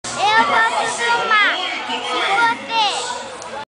1ª Caminhadão Max
A pequena Meiri Ellen, de apenas 5 anos, mostrou seu apoio ao prefeito Max.
By ValériaCamp